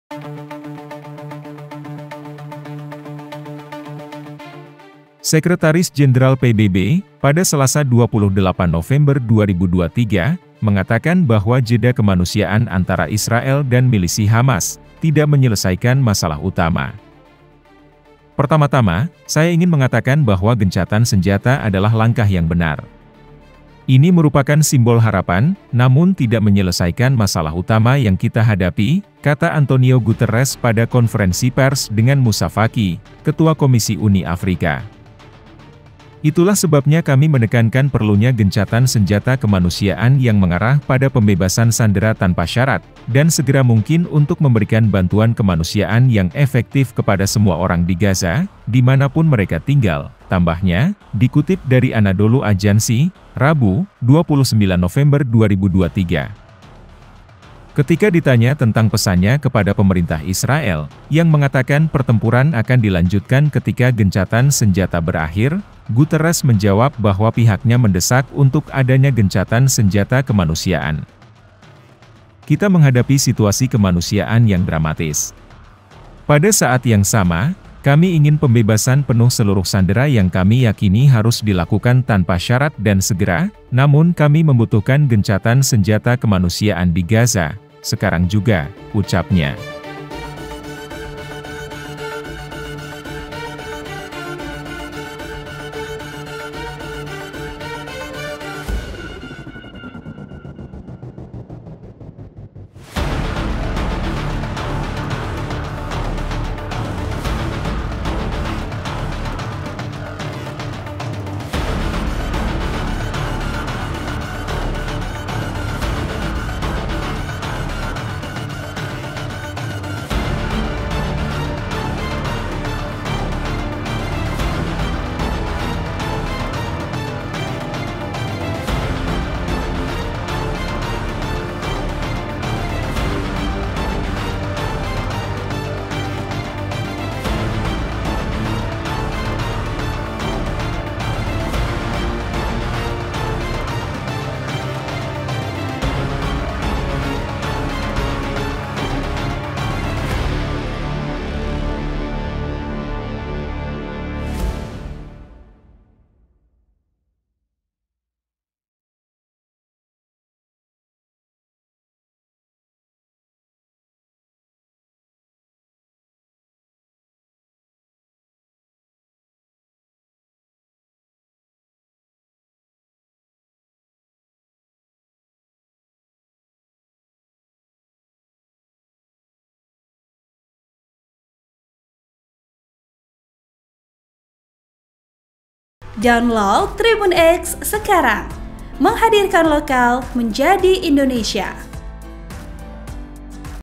Sekjen PBB Sebut Jeda Kemanusiaan di Gaza Tidak Selesaikan Masalah Utama
POS-KUPANG.COM - Sekretaris Jenderal PBB, pada Selasa 28 November 2023, mengatakan bahwa jeda kemanusiaan antara Israel dan milisi Hamas, tidak menyelesaikan masalah utama.

“Pertama-tama, saya ingin mengatakan bahwa gencatan senjata adalah langkah yang benar. Ini merupakan simbol harapan, namun tidak menyelesaikan masalah utama yang kita hadapi,” kata Antonio Guterres pada konferensi pers dengan Moussa Faki, ketua Komisi Uni Afrika.

“Itulah sebabnya kami menekankan perlunya gencatan senjata kemanusiaan yang mengarah pada pembebasan sandera tanpa syarat, dan segera mungkin untuk memberikan bantuan kemanusiaan yang efektif kepada semua orang di Gaza, di mana pun mereka tinggal,” tambahnya, dikutip dari Anadolu Ajansi, Rabu, 29 November 2023.

Ketika ditanya tentang pesannya kepada pemerintah Israel, yang mengatakan pertempuran akan dilanjutkan ketika gencatan senjata berakhir, Guterres menjawab bahwa pihaknya mendesak untuk adanya gencatan senjata kemanusiaan.

“Kita menghadapi situasi kemanusiaan yang dramatis. Pada saat yang sama, kami ingin pembebasan penuh seluruh sandera yang kami yakini harus dilakukan tanpa syarat dan segera, namun kami membutuhkan gencatan senjata kemanusiaan di Gaza, sekarang juga,” ucapnya.

Sebelumnya, Qatar mengumumkan perjanjian pada Senin malam, 27 November 2023, untuk memperpanjang jeda kemanusiaan selama dua hari tambahan, yang mana pertukaran tahanan akan dilakukan lebih banyak. Perjanjian itu disetujui oleh kedua belah pihak.

Artikel: viva.co.id
Vp: Dhev